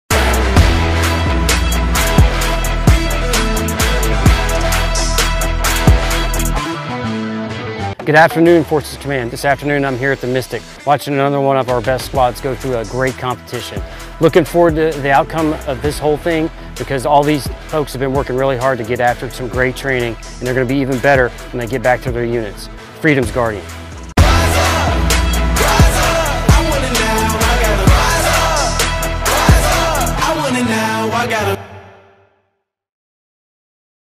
FORSCOM CSM Talks Best Squad Competition Day 3
FORT HOOD, Texas - U.S. Army Command Sgt. Maj. Todd Sims, the Command Sergeant Major of U.S. Army Forces Command, gives an update for day 2 of the U.S. Army Forces Command Best Squad Competition, at Fort Hood, Texas, August 16, 2022.

FORT HOOD, TX, UNITED STATES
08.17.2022

The Best Squad Competition reinforces fundamental Soldier skills and squad level tactics. (U.S. Army video by Sgt. James Alegria)

Film Credits: Video by Spc. James Alegria and Spc. James Liker
7th Mobile Public Affairs Detachment